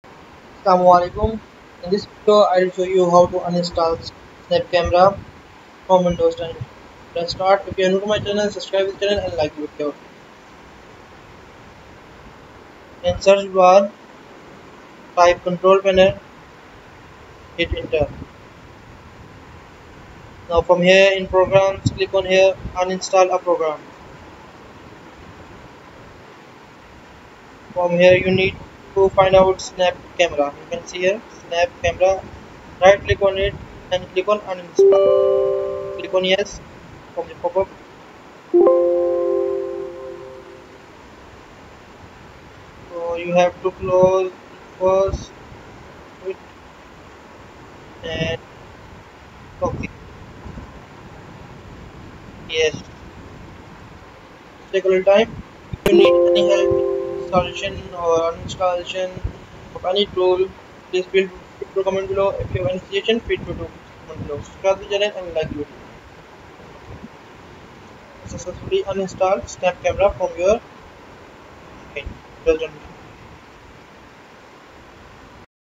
how to uninstall snap camera windows 10
Hi, In this video, I will show you how to uninstall snap camera windows 10